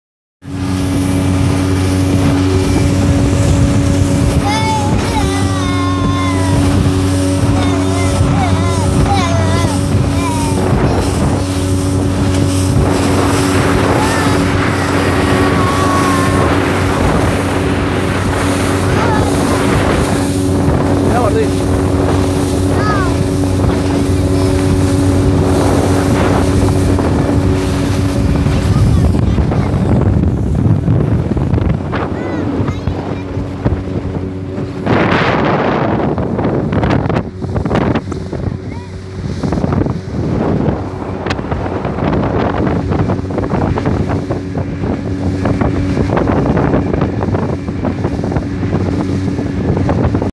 De paseo en lancha
Padre e hijo de paseo en lancha en montevideo